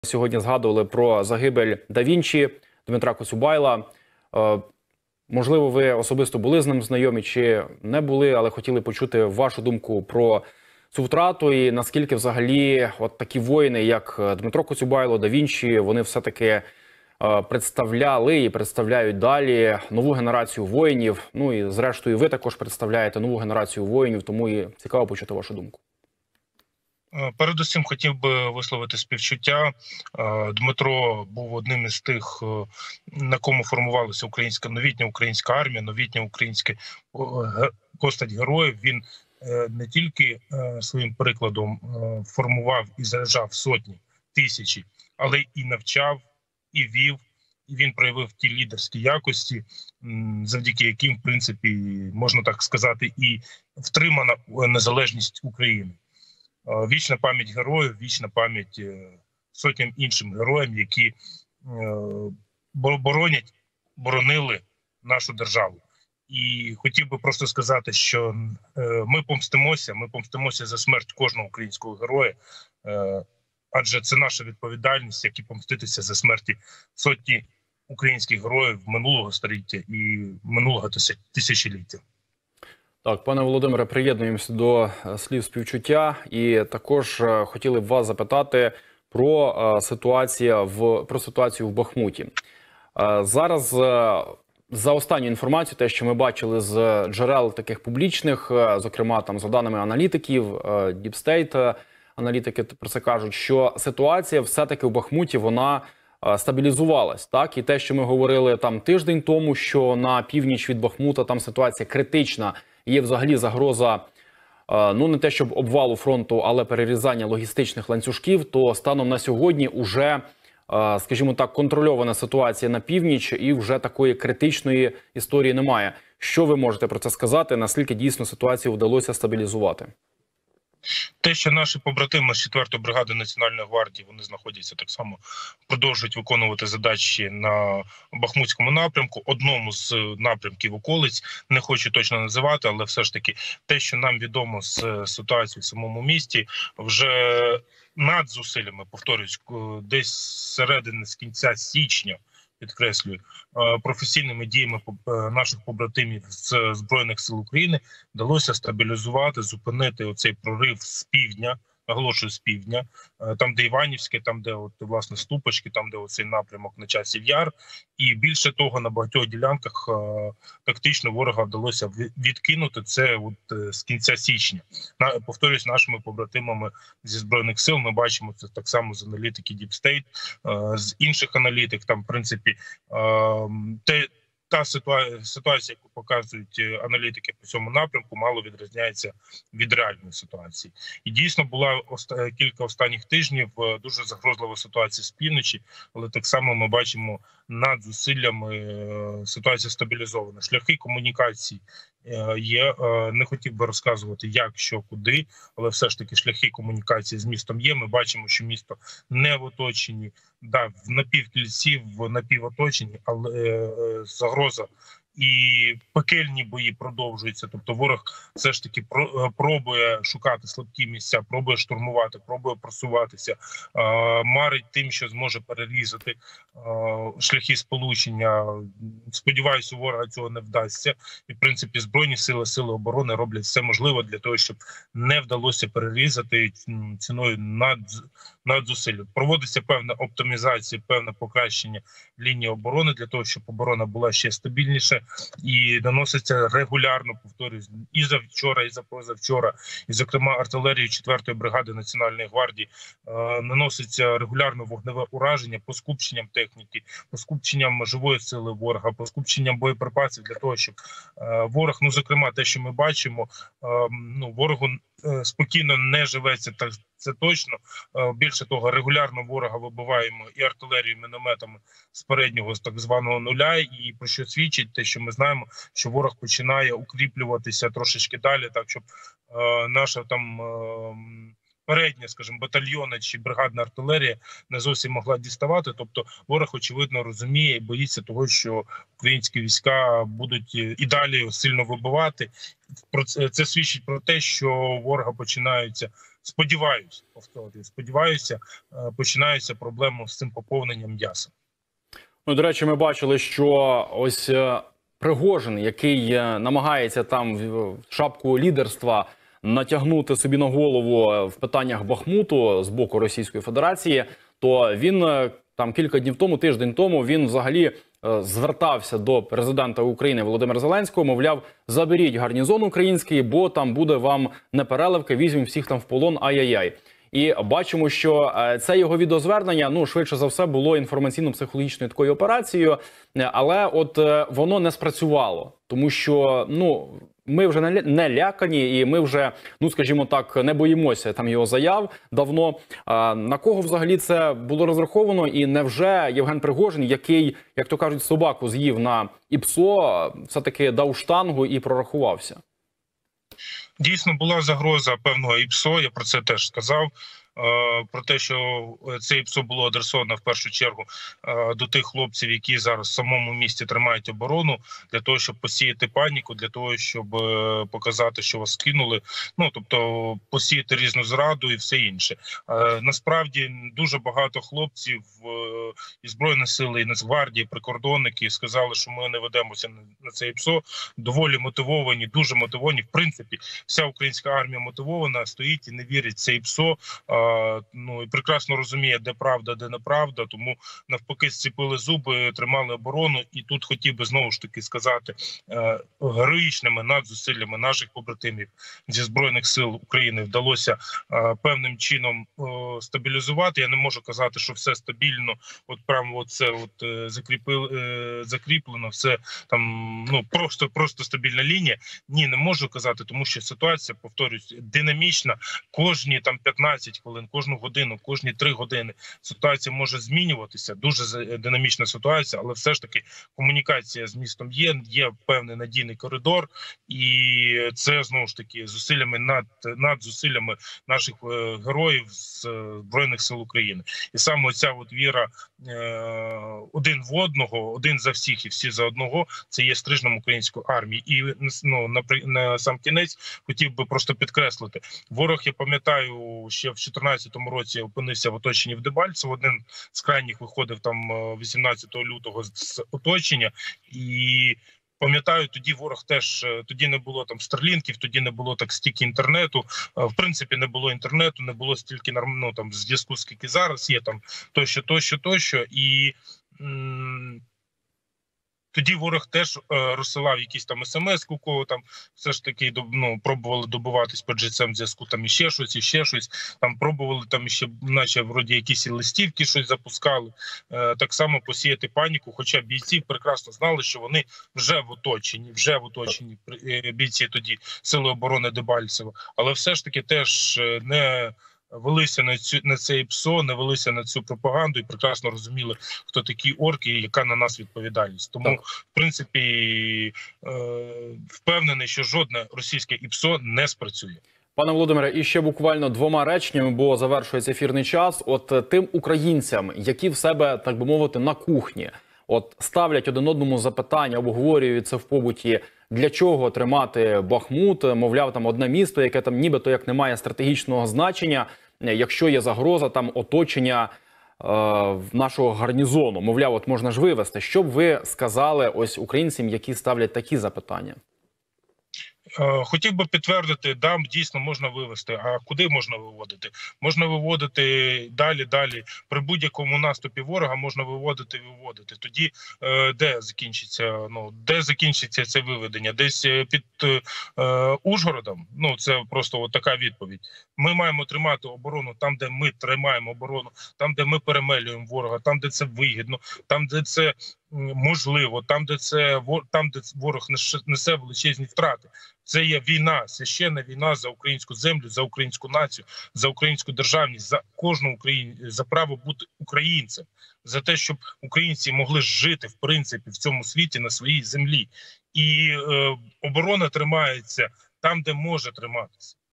Бахмут: загибель Да Вінчі, ІПСО ворога, шкода від диванних експертів — Володимир Назаренко з фронту
З Бахмутського напрямку —  Володимир Назаренко, заступник командира 3-го батальйону «Свобода» у складі Бригади швидкого реагування Національної гвардії України «Рубіж» в етері телеканалу «Апостроф ТБ» вранці 8 березня 2023 року